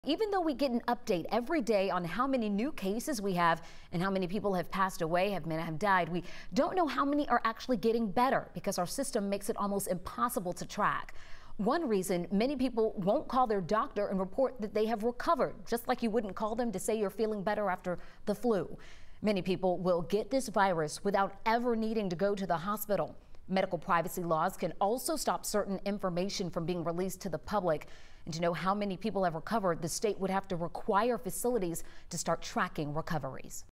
Why the state doesn't track COVID-19 recoveries
Many won't call their doctor and report that they have recovered.